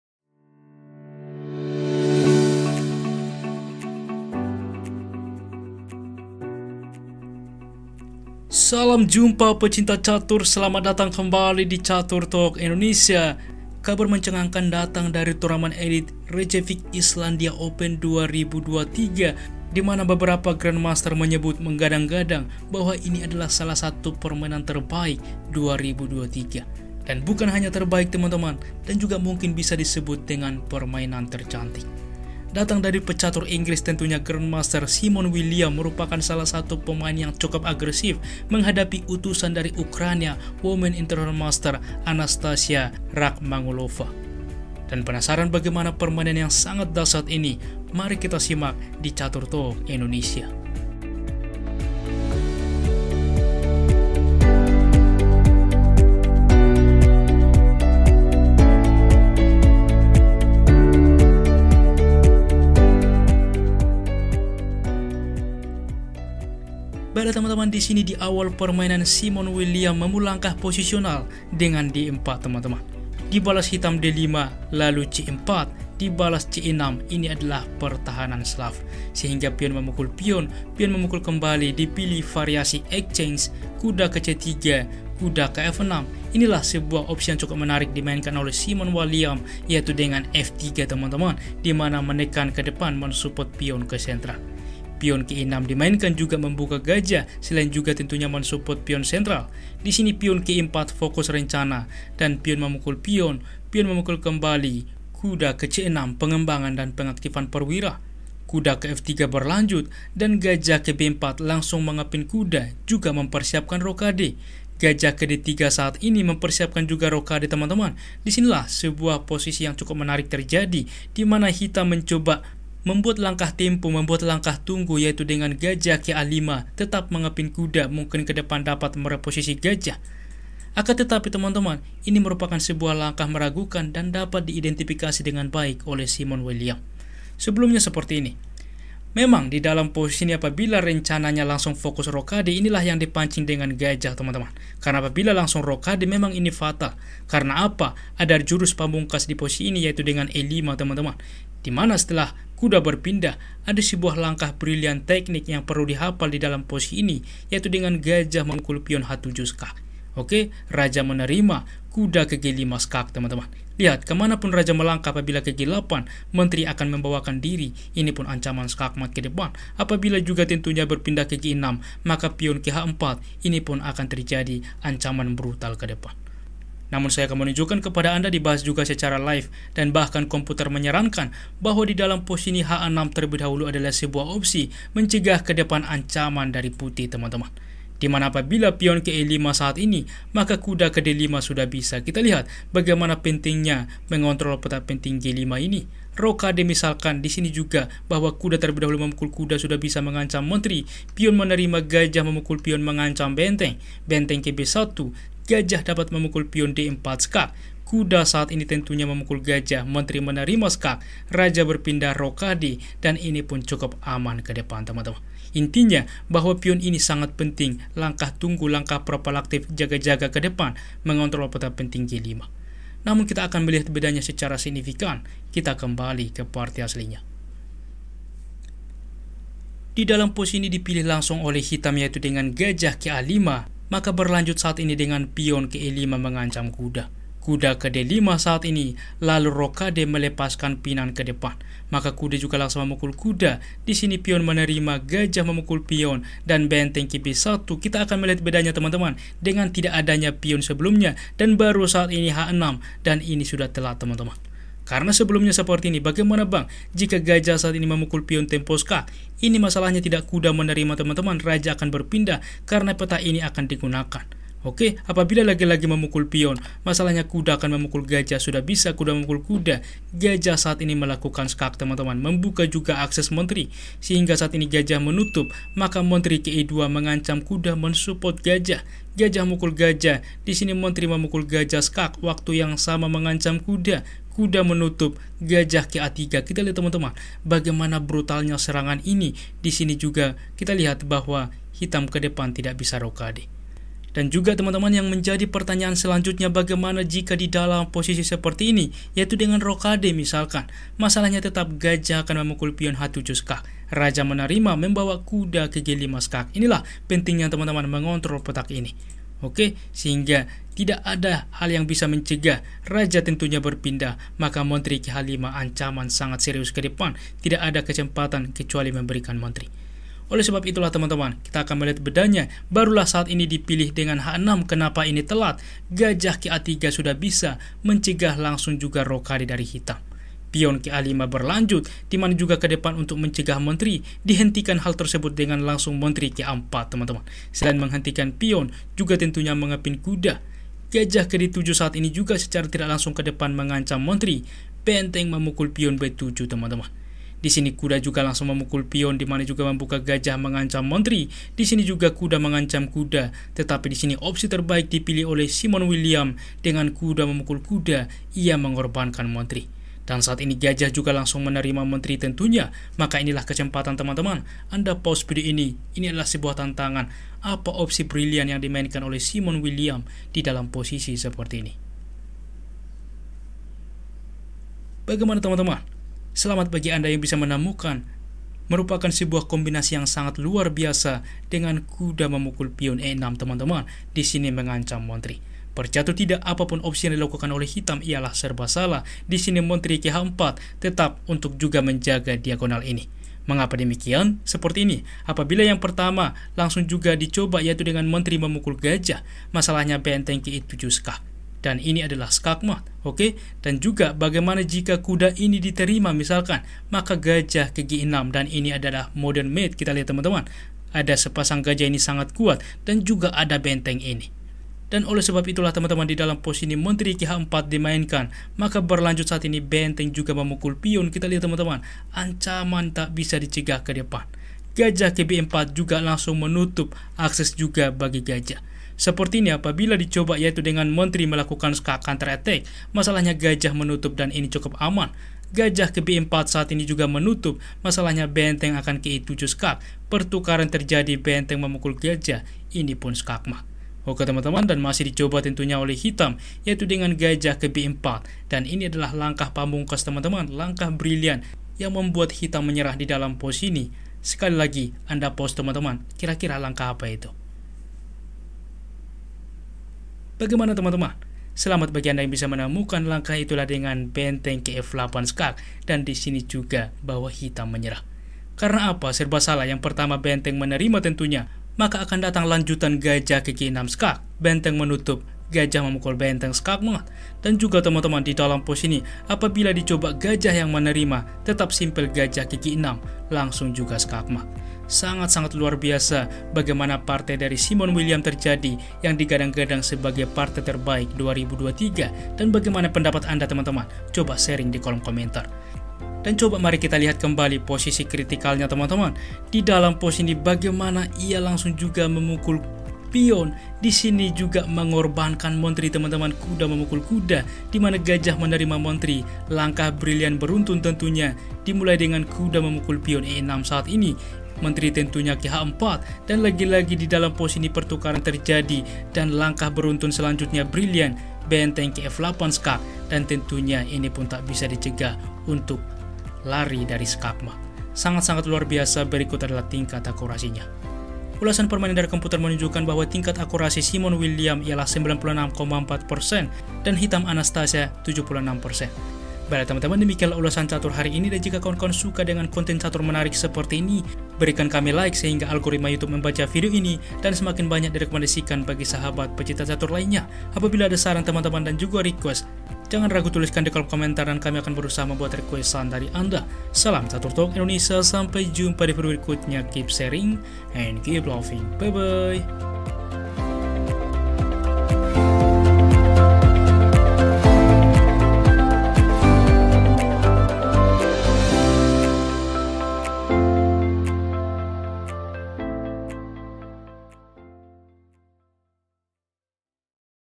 PARTAI TERBAIK 2023 TELAH LAHIR 🎉
PGN Game ! Simon William Vs Anastasiya Rakhmangulova 2023
[Event "Reykjavik Open 2023"]
[Site "Chess.com"]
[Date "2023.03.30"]
[Round "02"]
[White "Williams, Simon K"]
[Black "Rakhmangulova, Anastasiya"]
[WhiteElo "2461"]
[BlackElo "2266"]
[TimeControl "40/5400+30:1800+30"]

1. d4 d5 2. c4 c6 3. cxd5 cxd5 4. Nc3 Nf6 5. f3 e6 6. e4 dxe4 7. fxe4 Nc6 8. Nf3
Bb4 9. Bd3 Ba5 10. e5 Nd5 11. O-O Nxc3 12. bxc3 Bxc3 13. Rb1 h6 14. Ba3 a5 15.
Qa4 Bd7 16. Rxb7 Nxd4 17. Nxd4 Bxa4 18. Nxe6 Qh4 19. Rfxf7 Bb4 20. Rf8+